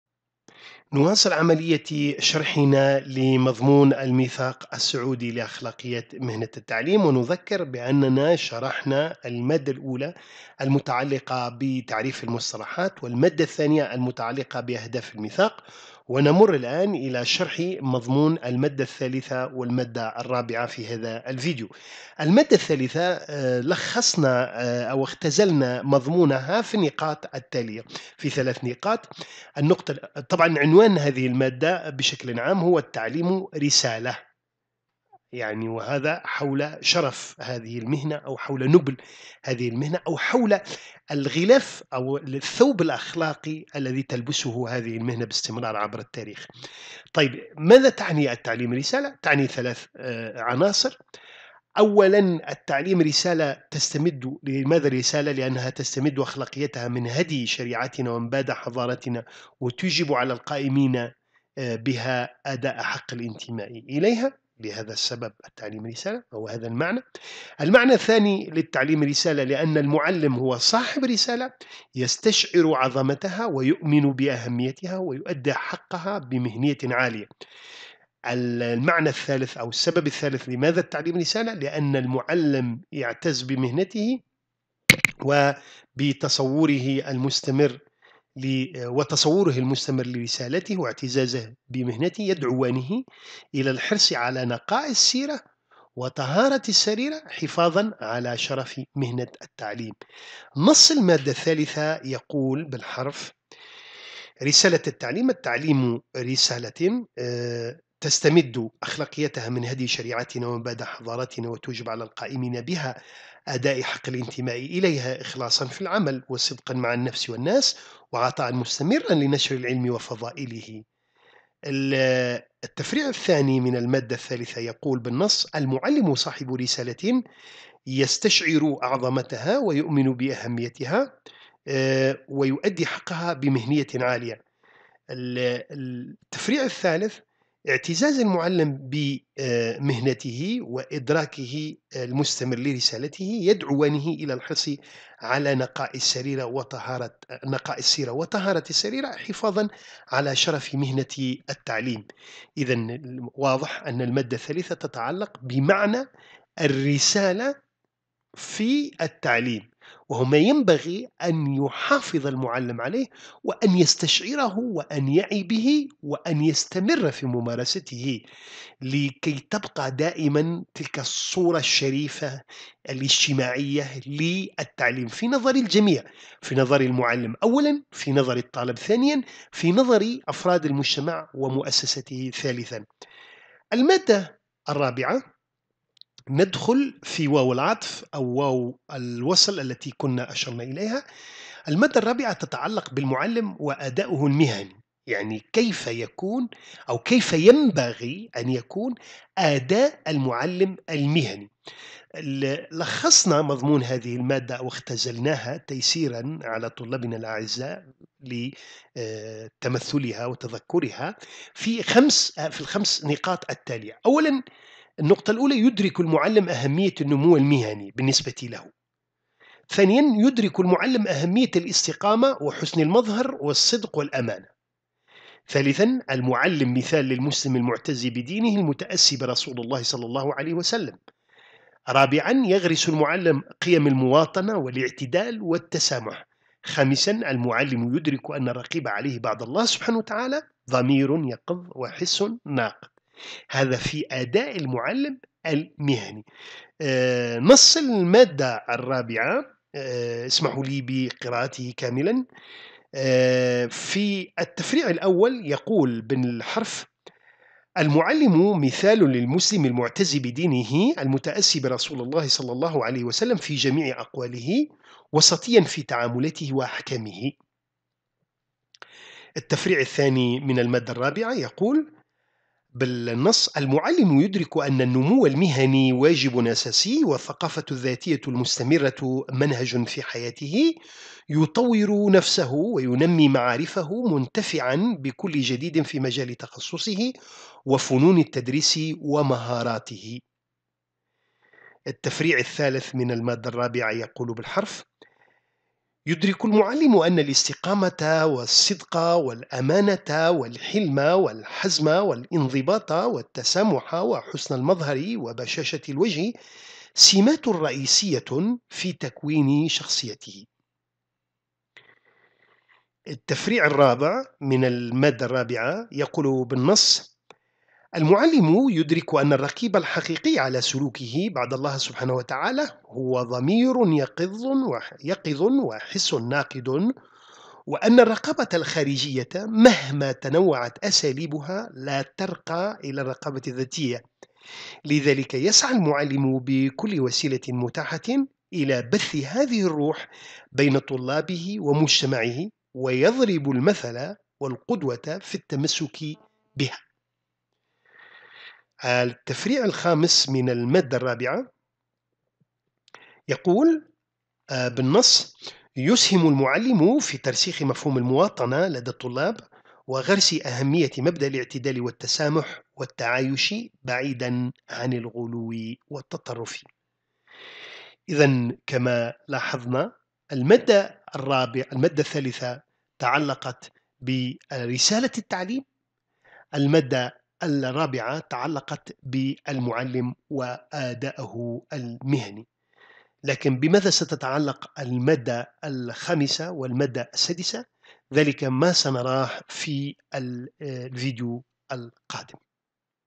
أخلاقيات المهنة | 4-4 | الميثاق السعودي لأخلاقيات مهنة التربية والتعليم : المادة الثالثة والرابعة
المقررات المفتوحة - أخلاقيات المهنة
الوحدة 4 - الدرس 4 : الميثاق السعودي لأخلاقيات مهنة التربية والتعليم : المادة الثالثة والرابعة
الميثاق السعودي لأخلاقيات مهنة التربية والتعليم:
ـ التعليم رسالة
المعلم وأداؤه المهني